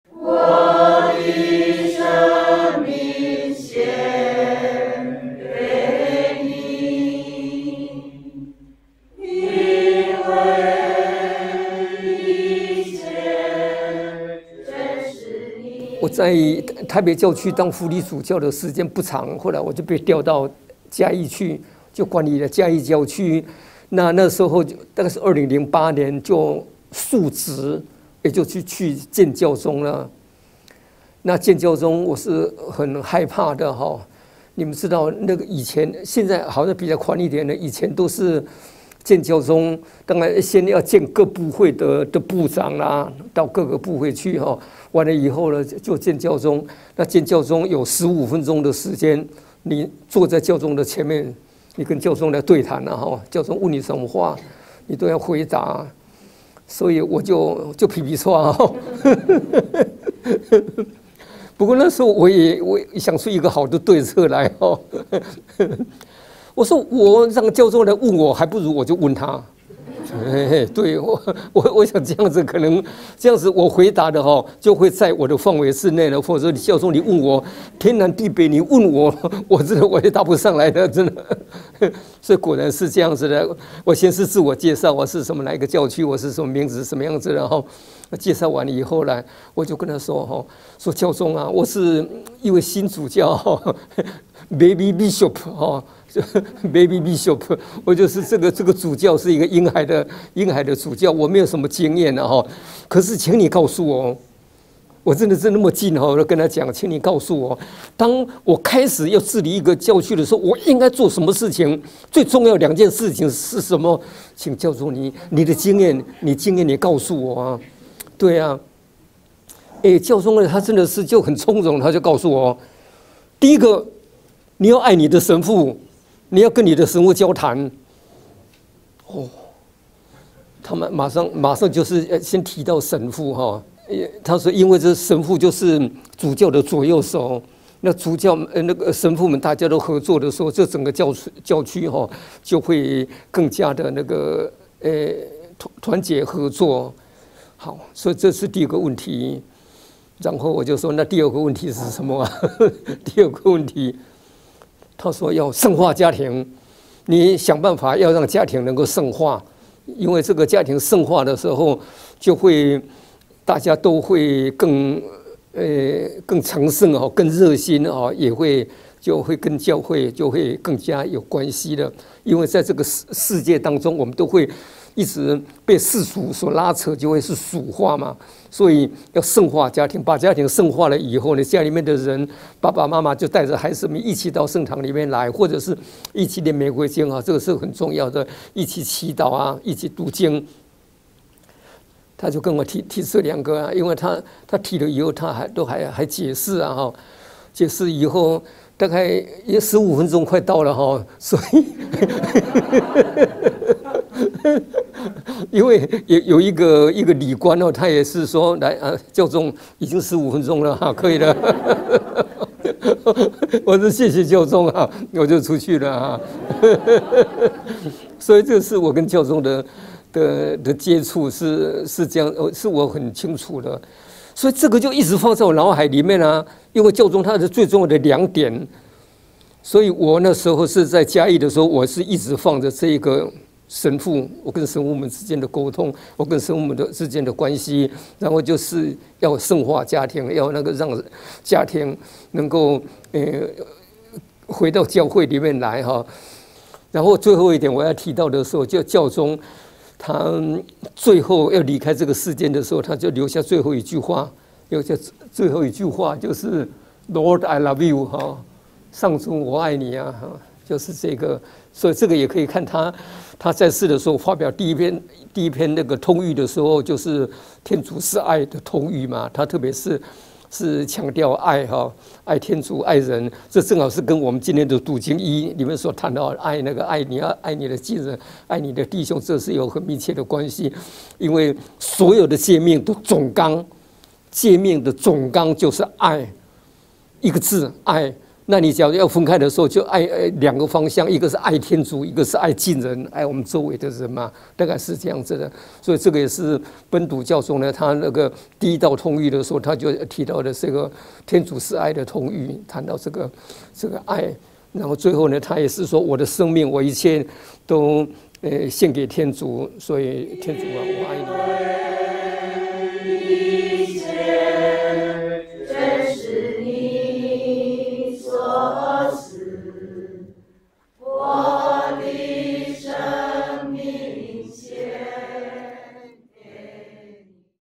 【與教宗15分鐘的對談】教宗說了什麼？｜榮休教宗本篤十六世
1月5日下午4點，在台北總主教公署舉行榮休教宗本篤16世的安息彌撒，鍾總主教分享自己曾經和教宗的15分鐘談話，提及這場對談是如何影響他治理一個教區。